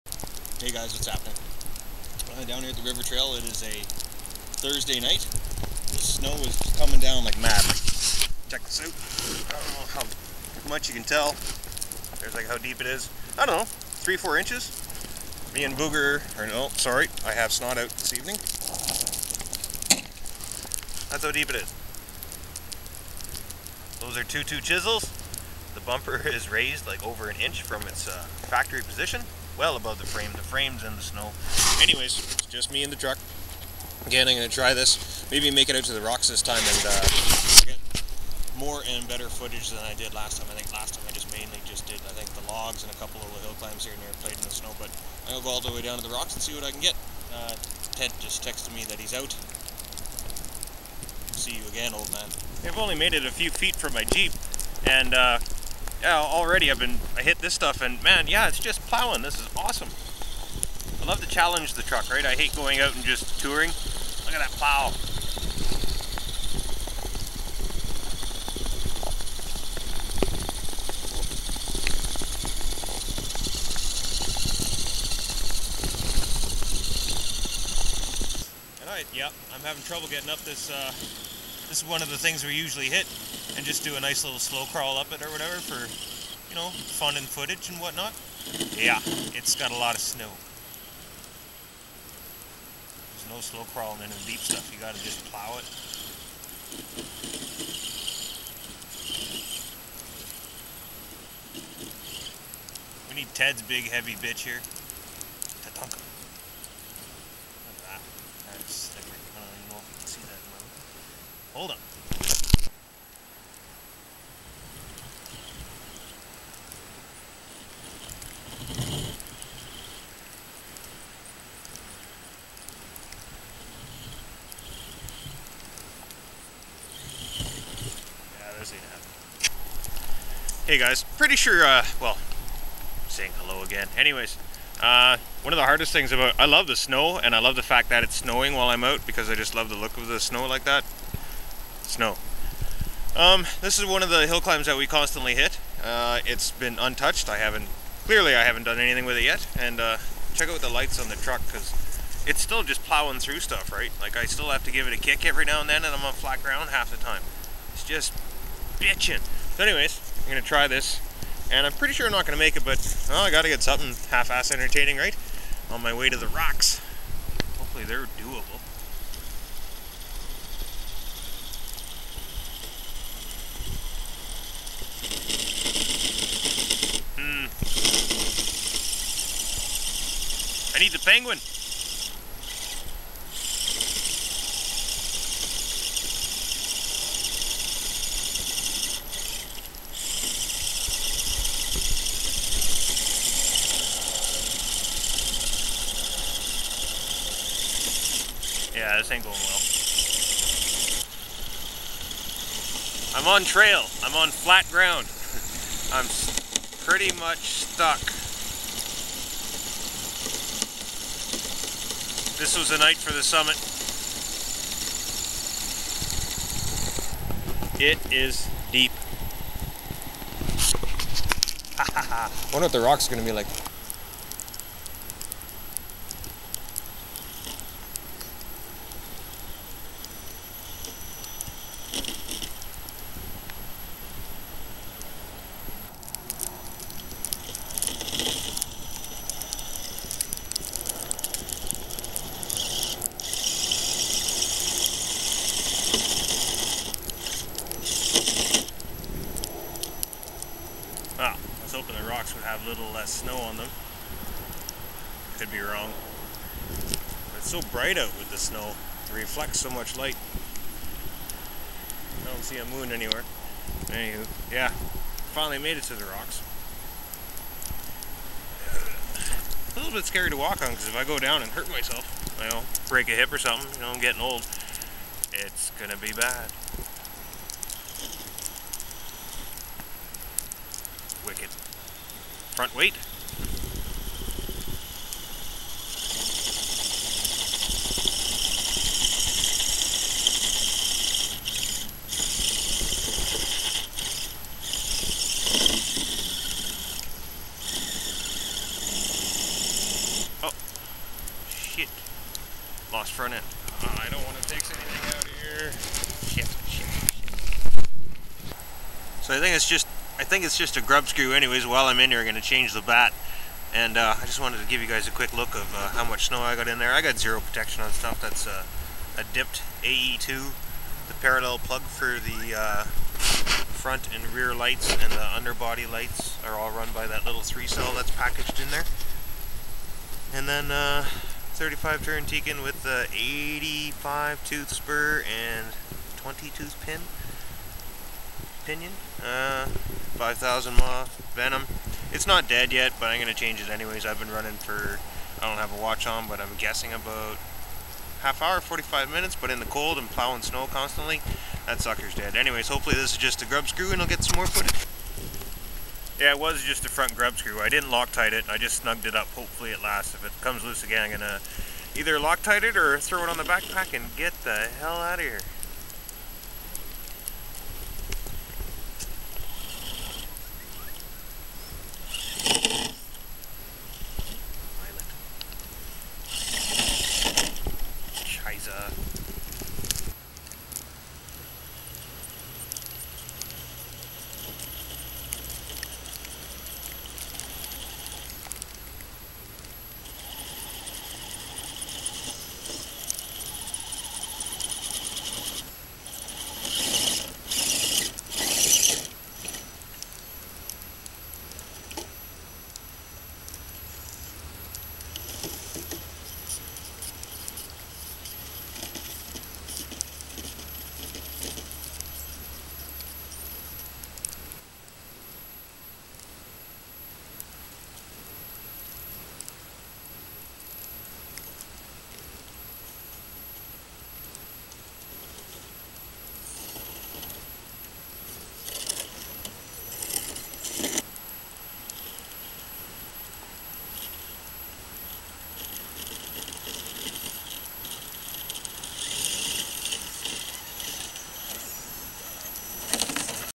RC ATHLETES - In some REALLY Deep SNOW!!
Just me and snot again tonight. I almost had Ted out but he gave up on the idea because it was soo deep. Oh well, I had a good time :)
This was definetly summit territory tonight thogh. Was a little too deep lol.

ps -  I bought a drifter :)